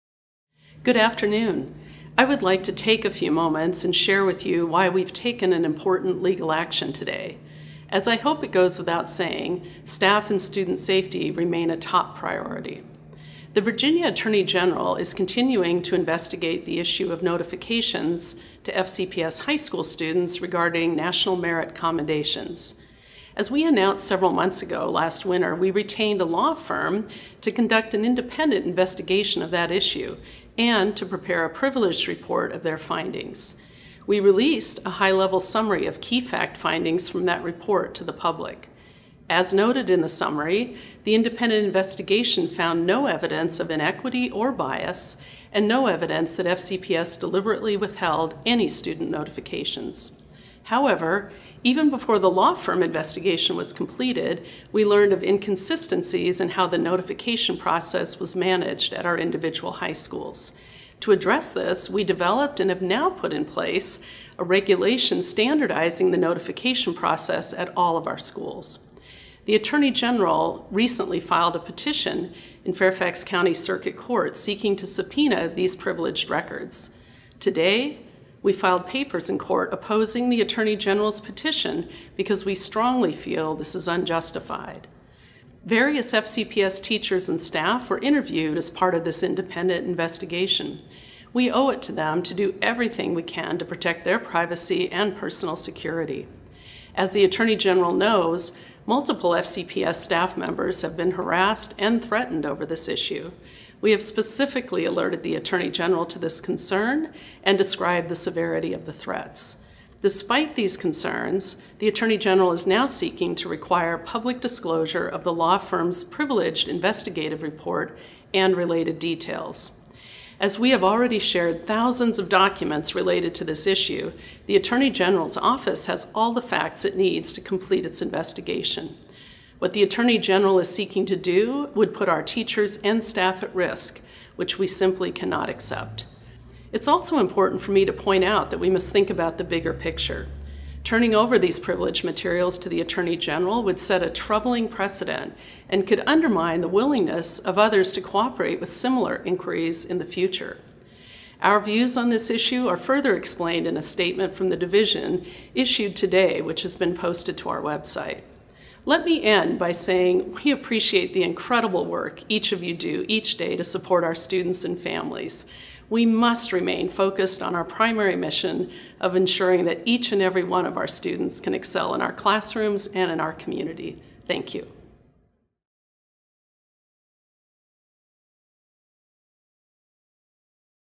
Fairfax County Public Schools responds to subpoena to release merit awards report
The school district says it's taken "legal action" to fight the subpoena.